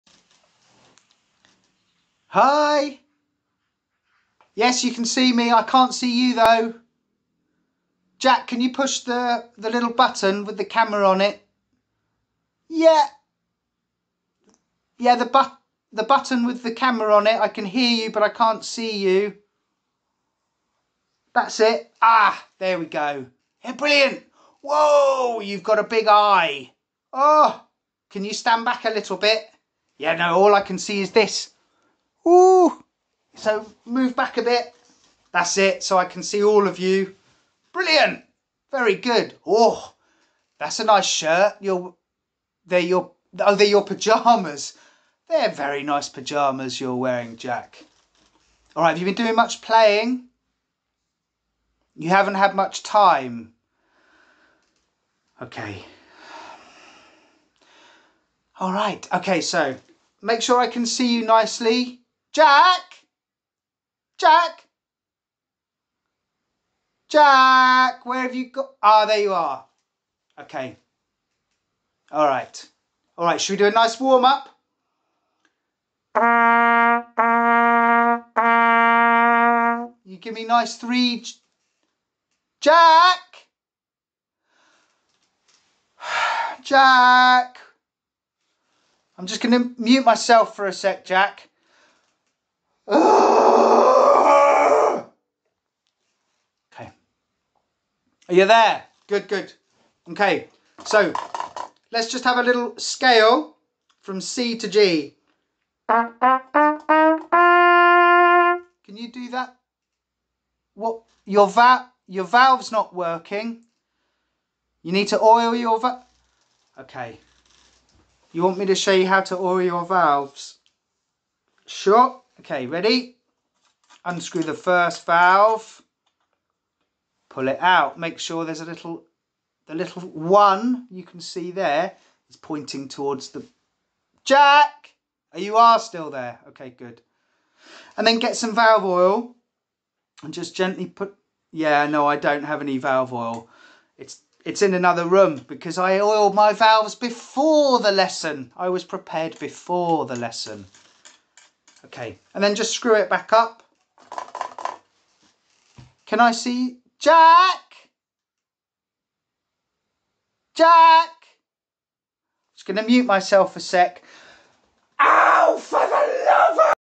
Online video lessons be like.....
A representation of an online trumpet lesson